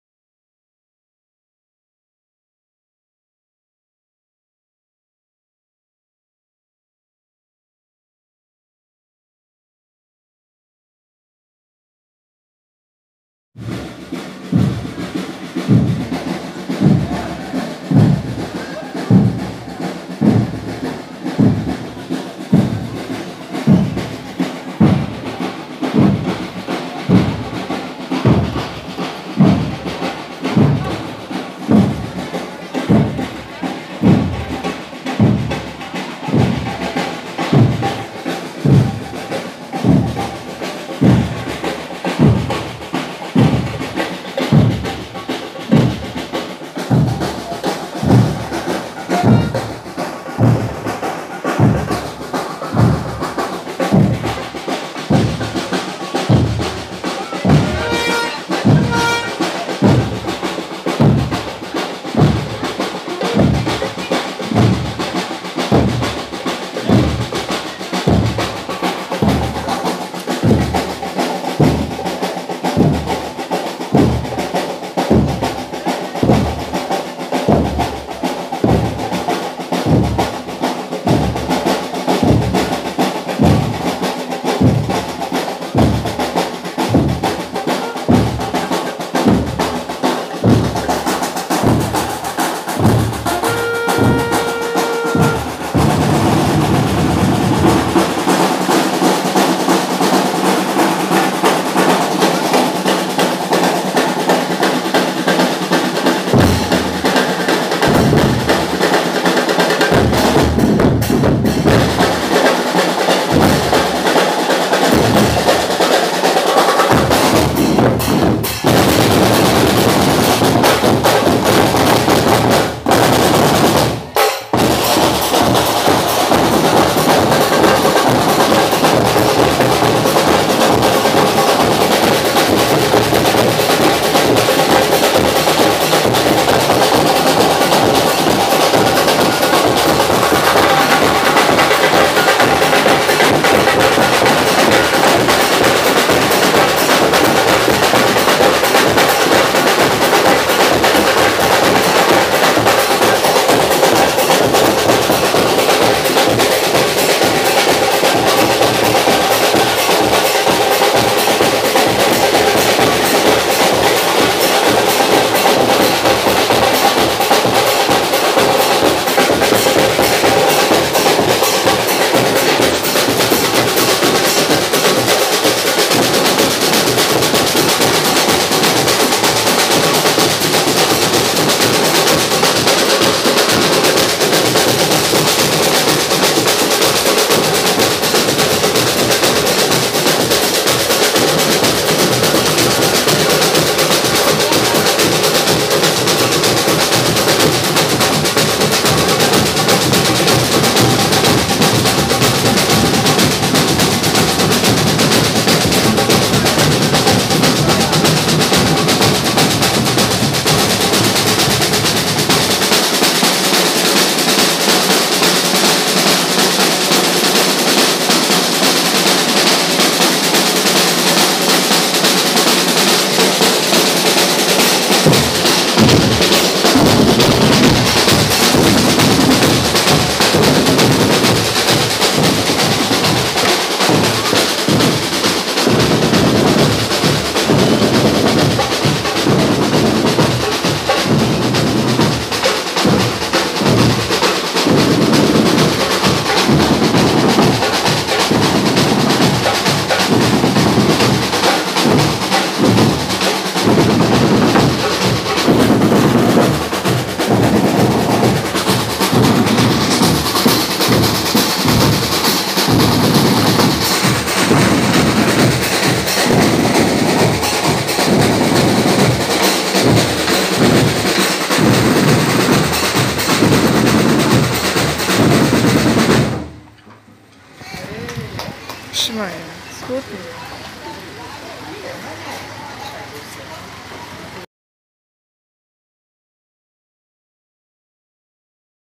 Desfile 7 de setembro em F. Velho
Fiz esse pequeno vídeo para poder compartilhar com vcs um pouco do desfile Cívico que aconteceu no meu bairro aqui na cidade de Maceió.
Que Deus abençoe a todos, bjos.👍😘